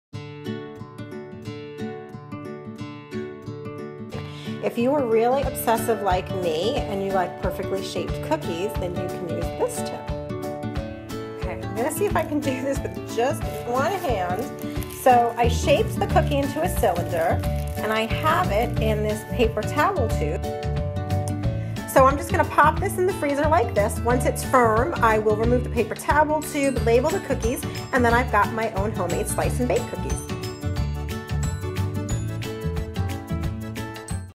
How to Make Perfect Slice & Bake Cookies
All you need is an empty paper towel roll to make perfect slice & bake cookies every time. Watch my trick in this snapchat recap video!